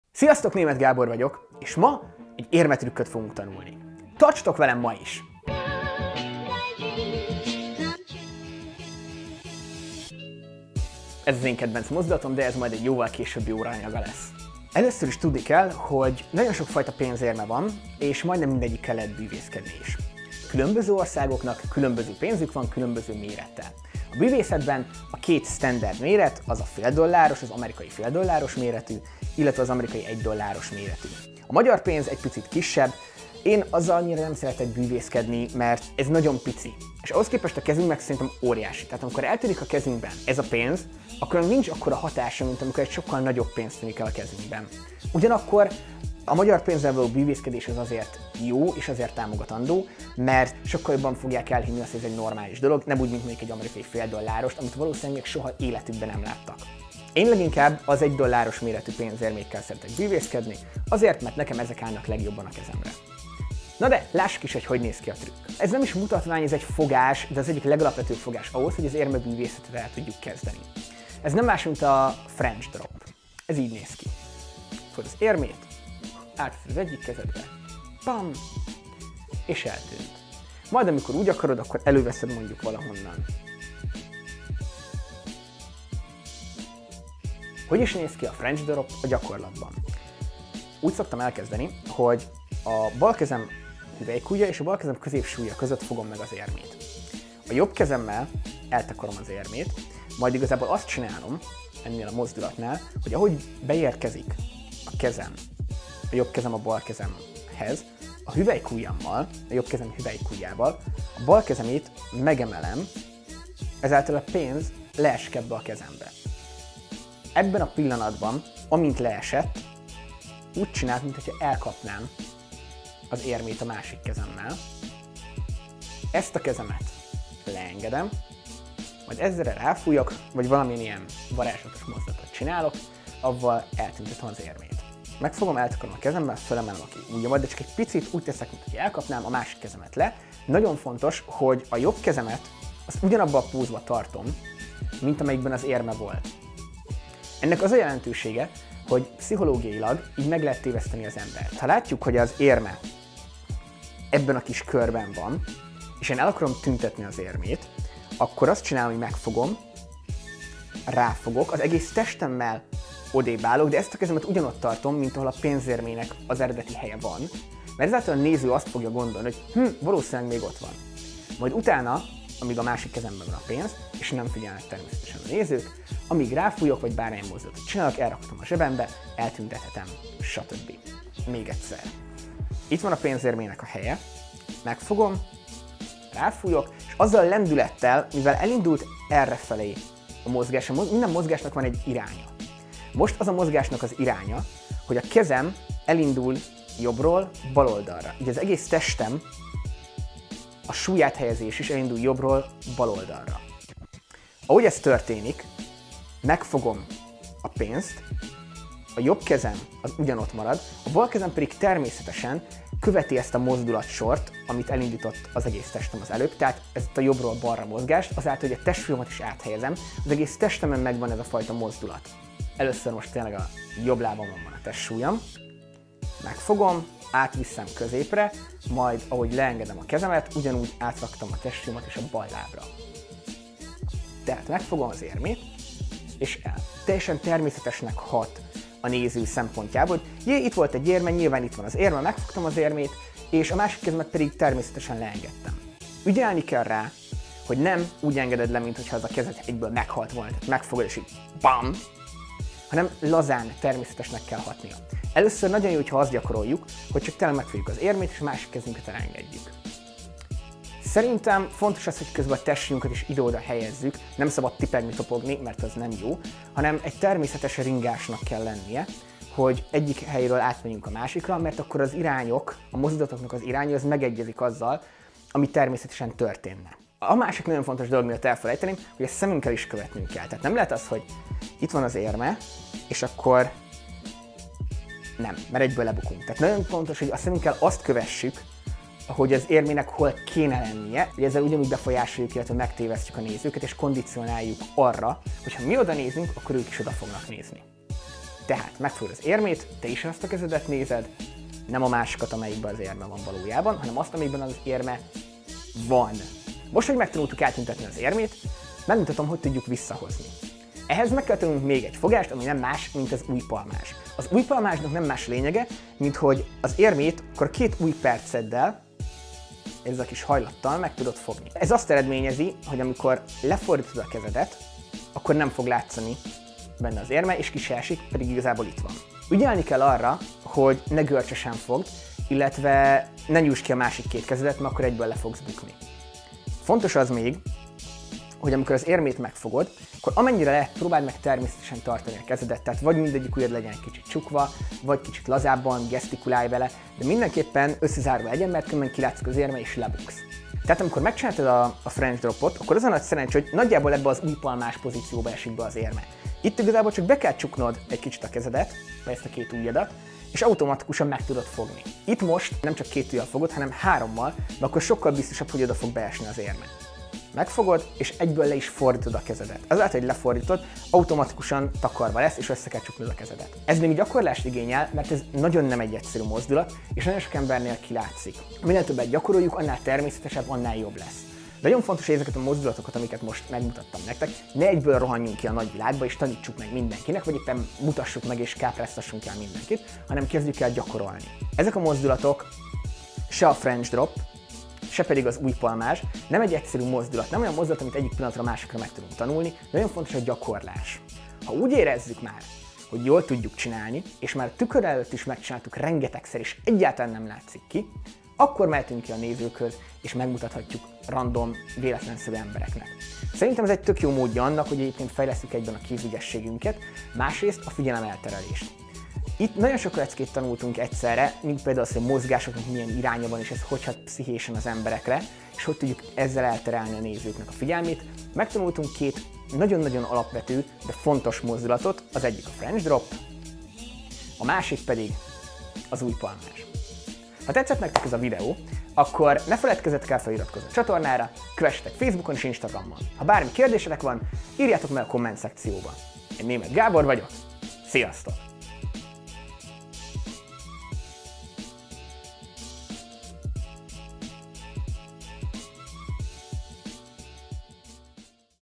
TRÜKK TANULÁS - Látványos érmetrükk kezdőknek (Németh Gábor Bűvész)
Ez a  Tanulj trükköt! Egy websorozat, ahol bűvésztrükköket tanítok nektek.

A mai részben egy  érmetrükköt fogtok megtanulni. Tartsatok velem ma is!